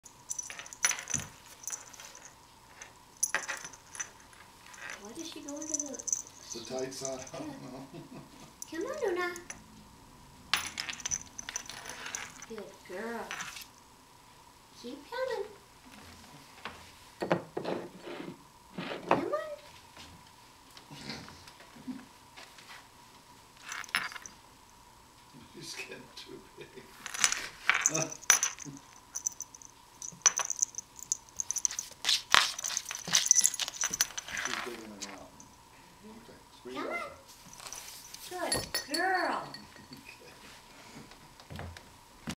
Copy of MVI 5217
Luna squeezes under door.